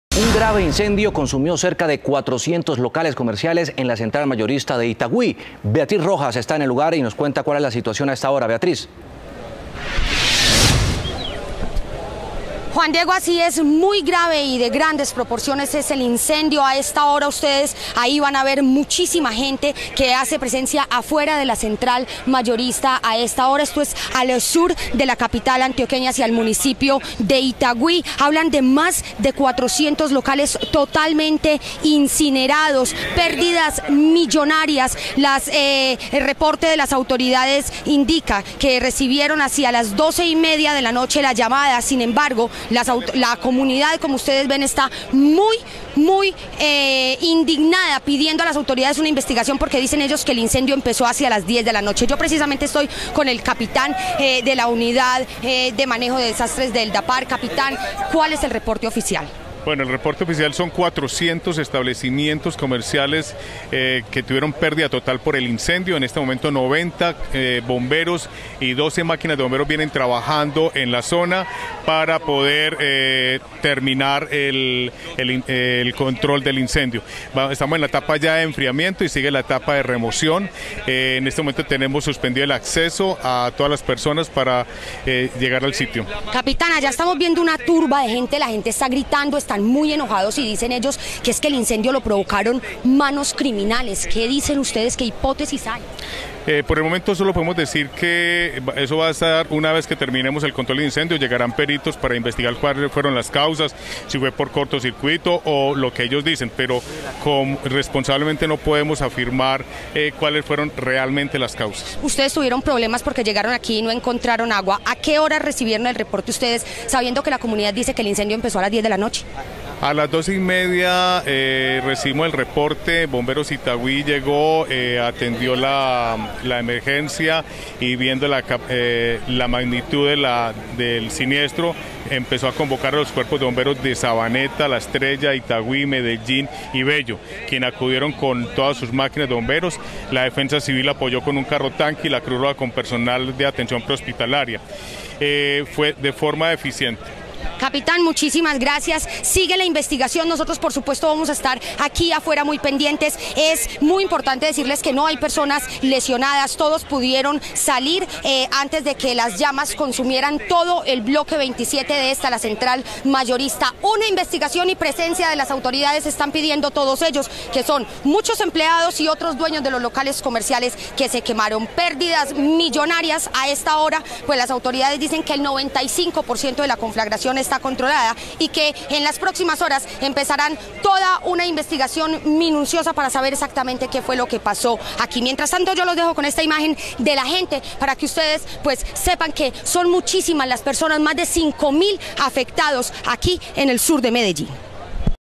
Grave incendio durante las horas de la madrugada dejó 400 locales comerciales destruidos en Itagüí
Según informaron las autoridades, la emergencia fue atendida por 90 bomberos y 12 máquinas que aún hacen presencia en el lugar de la conflagración.Pasada la medianoche del día domingo 9 de julio, la comunidad alertó sobre un incendio que se presentó en el bloque 27 de la Central Mayorista del municipio de Itagüí.En esta bodega actualmente funcionaban cerca de 400 establecimientos de aproximadamente tres metros cuadrados.Al lugar hicieron presencia miembros del equipo de bomberos provenientes de Bello, Itagüí, La Estrella, Envigado, Sabaneta y Medellín.Así mismo personal de la Defensa Civil y Cruz Roja atendieron la emergencia.Por el momento las autoridades investigan las posibles causas que habrían generado esta fuerte conflagración que dejaría pérdidas millonarias.